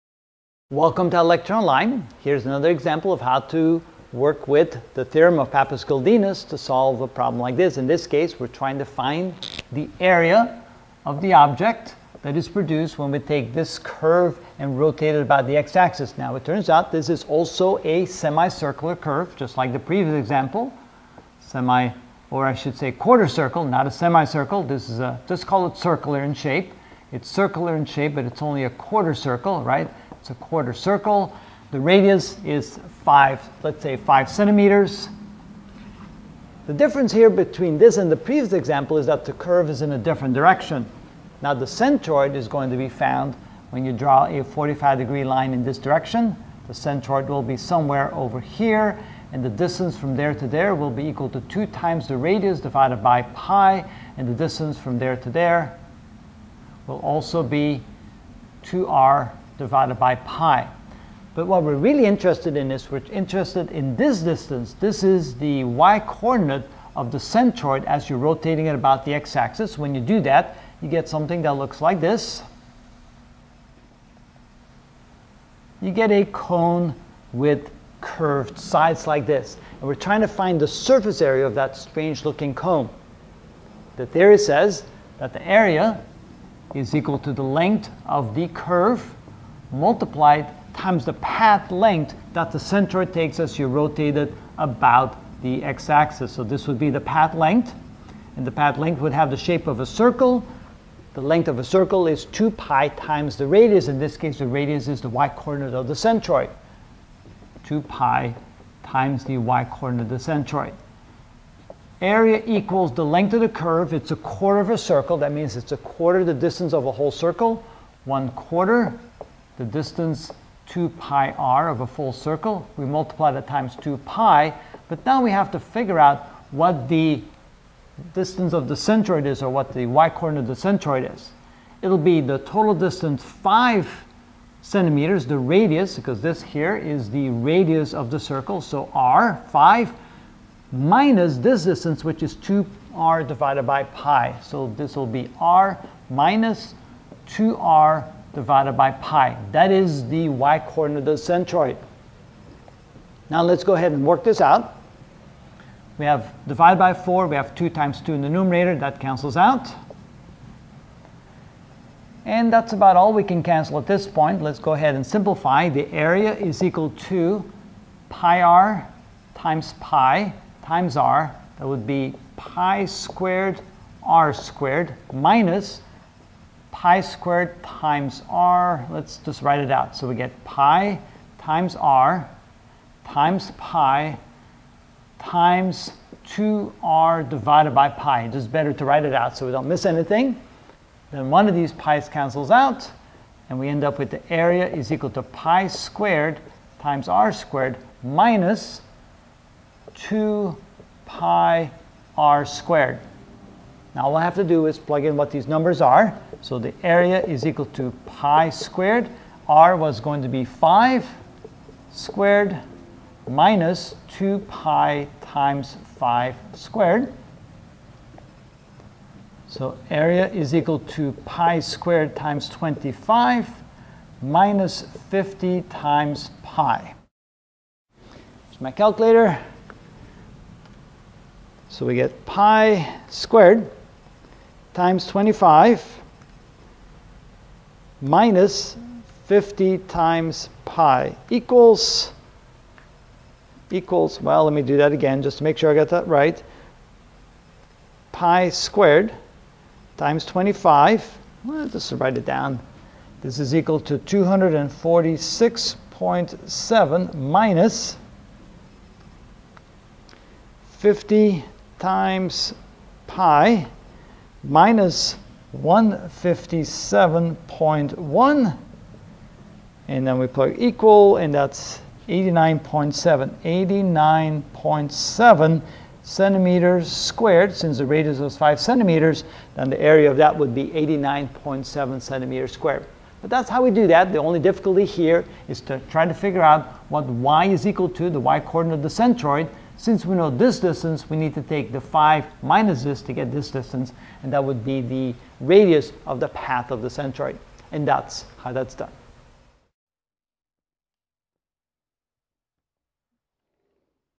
Mechanical Engineering: Centroids & Center of Gravity (28 of 35) Area=? using Pappus-Guldinus
In this video I will find the area of a quarter circular curve from the origin rotated about the x-axis (hollow trumpet).